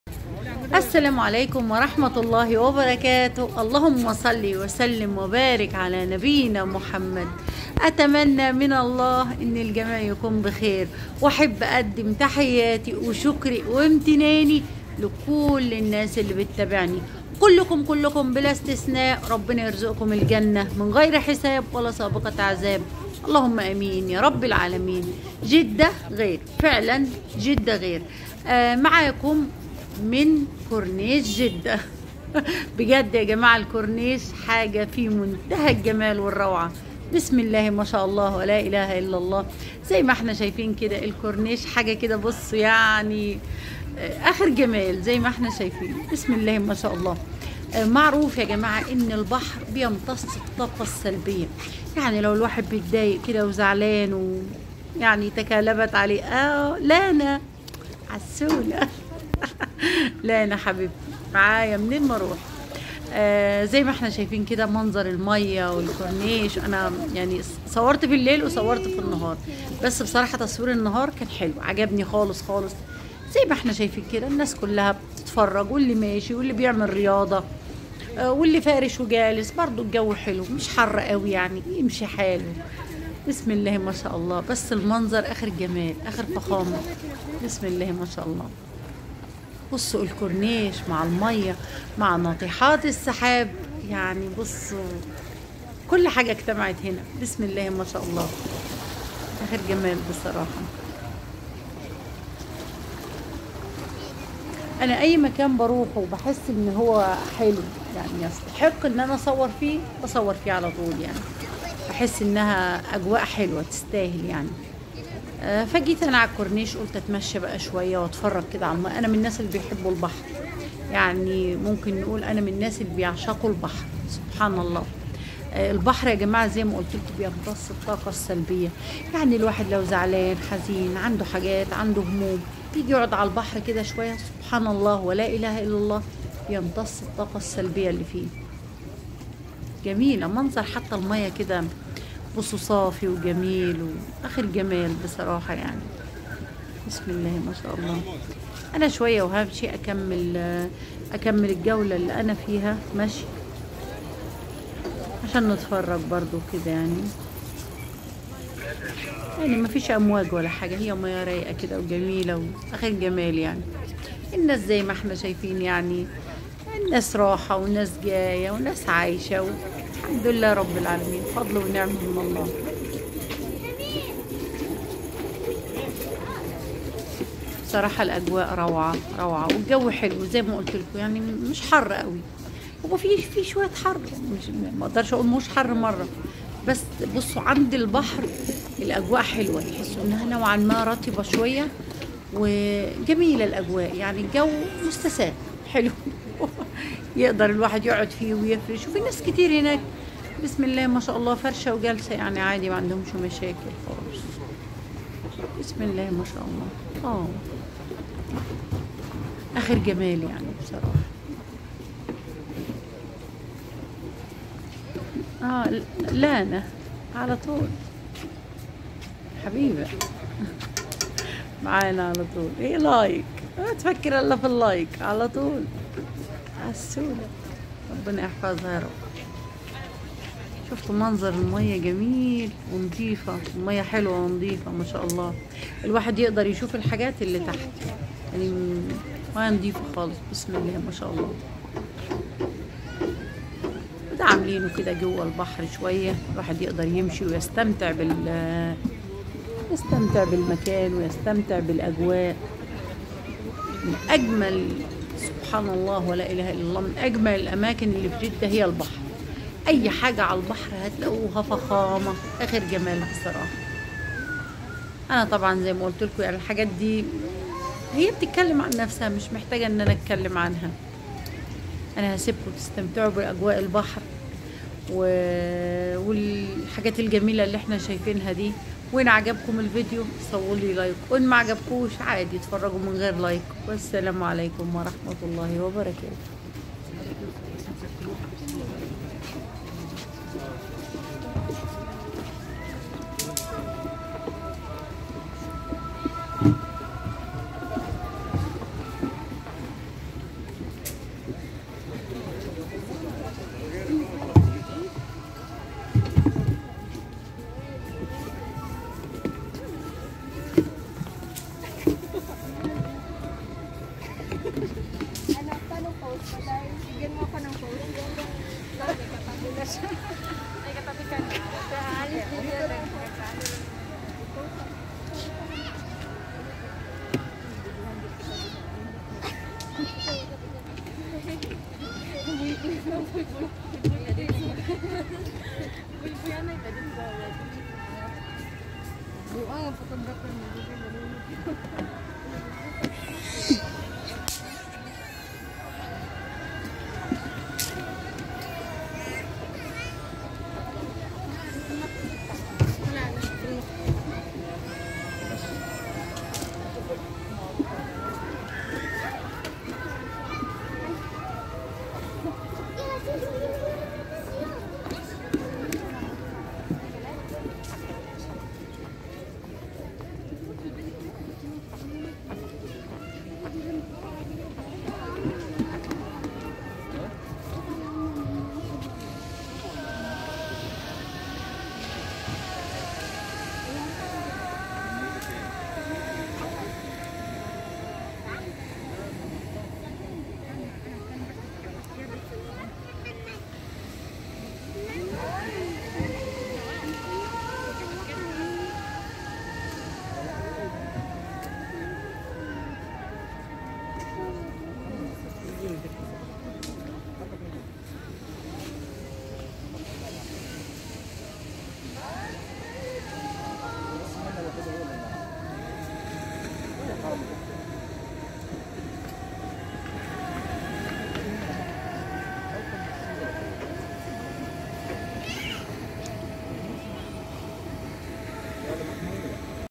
يوميات_زمن_الطيبين_فى_جده🇸🇦🇸🇦🇸🇦كورنيش_جده🌺🇸🇦🇸🇦🇸🇦🇸🇦🌺
كورنيش جده الجديد حيث الاجواء الرائعه والمناظر الخلابه والاجواء العليله  من اجمل الاماكن فى جده وقمه الروووووووعه 🌺🇸🇦🇸🇦🌺